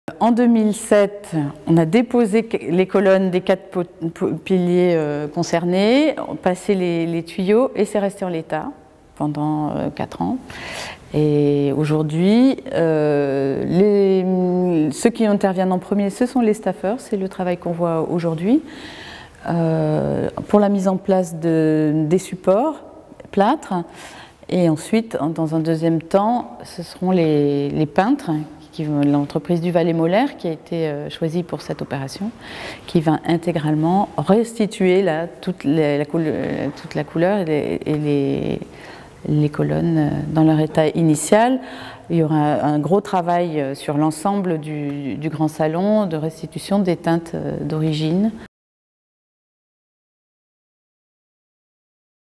Making-of | Restauration du grand salon de la Maison des étudiants de l'Asie du sud-Est [Partie 1]
Le grand salon de la Maison des étudiants de l'Asie du sud-Est est en restauration.
Le point sur l'actualité du chantier avec Charlotte PUEYO de l'agence AKPA ARCHITECTES qui nous parle de ce fleuron de la Cité internationale universitaire de Paris.
Réalisation et montage : Aurélien BARBIN